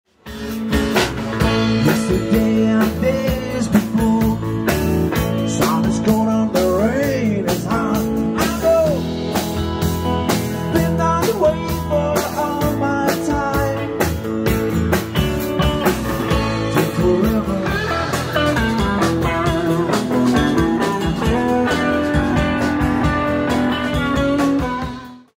The Mystery Train Rhodes ( Live at Electra Palace Hotel)demo
Mystery Train is a Music Band playing a mixture of Country , Rhythm n' Blues & Rock n’ Roll covers